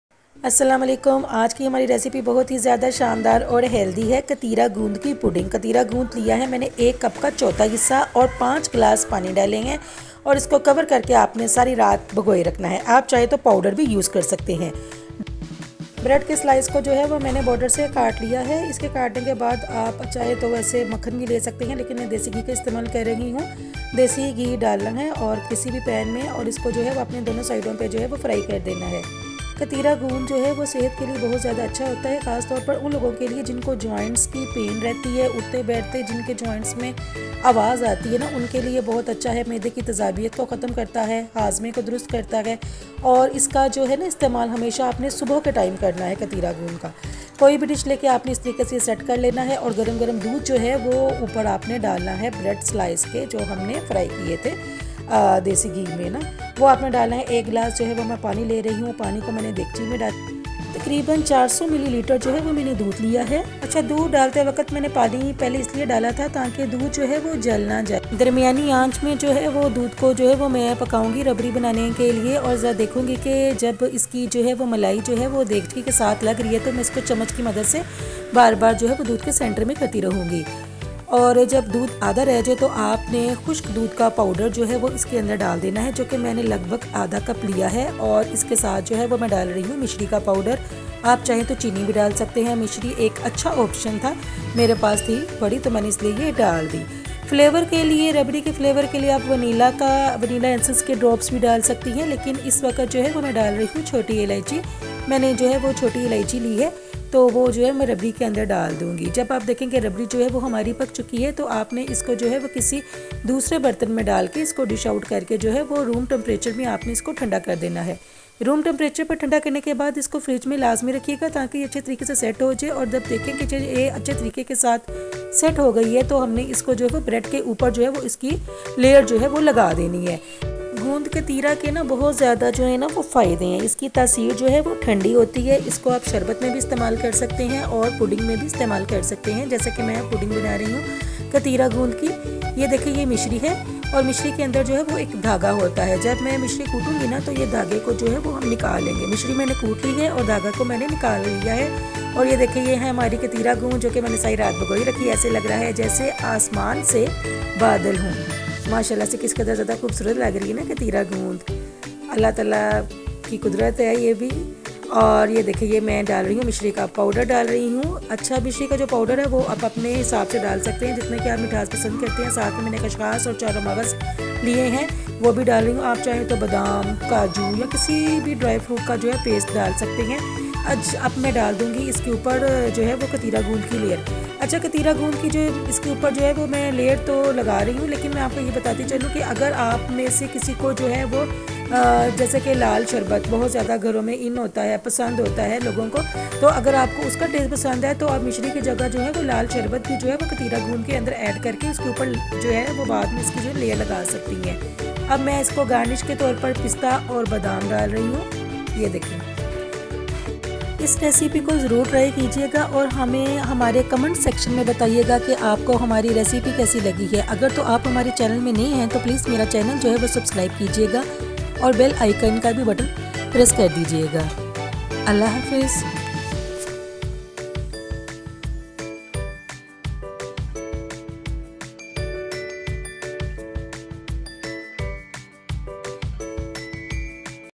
How to making KATIRA GOOND pudding | Summer Beat | Food Menu
Learn how to make a refreshing and delicious KATIRA GOOND pudding perfect for the summer season. This dessert is sure to be a hit on your food menu and will impress all your guests. Follow along with the easy steps in this video to create a sweet treat that everyone will love. Enjoy the cool and creamy texture of this dessert as you beat the heat this summer. Watch now to master this simple yet impressive recipe.